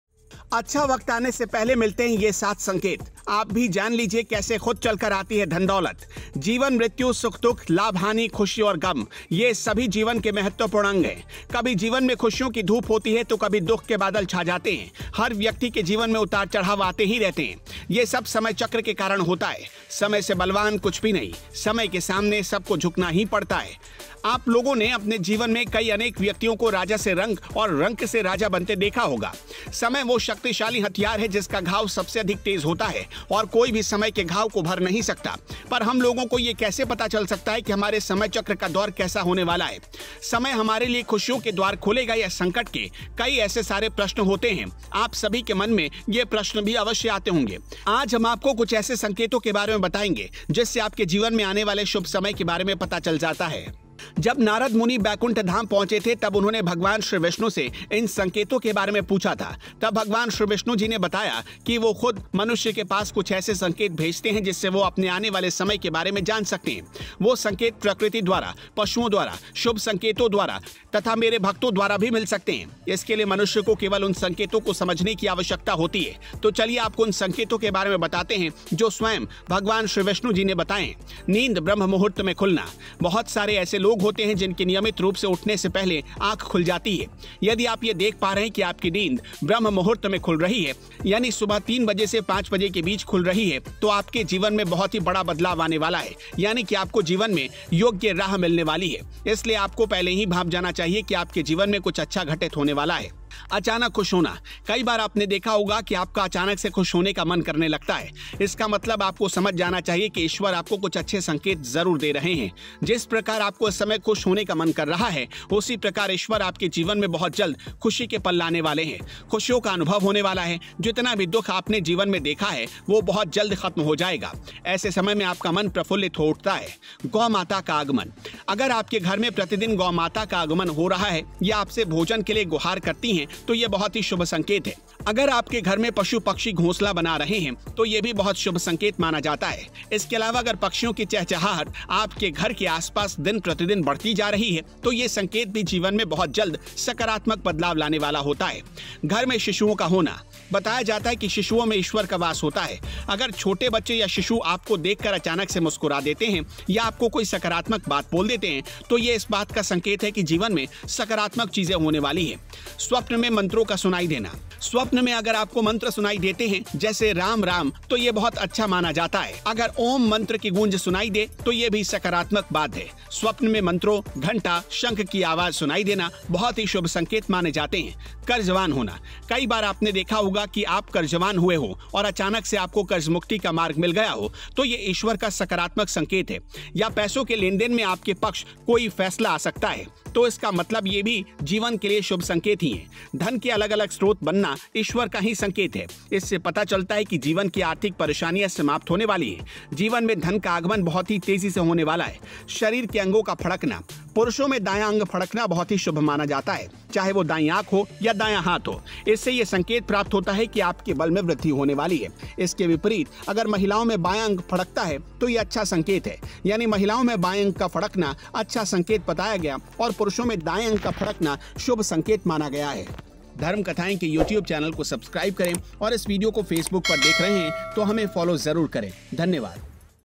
7 संकेत जो बताते हैं कि मनुष्य का अच्छा वक़्त आने वाला है | Lord Krishna Signs of upcoming Happiness
अच्छा वक्त आने से पहले मिलते हैं ये 7 संकेत, आप भी जान लीजिए कैसे खुद चलकर आती है धन-दौलत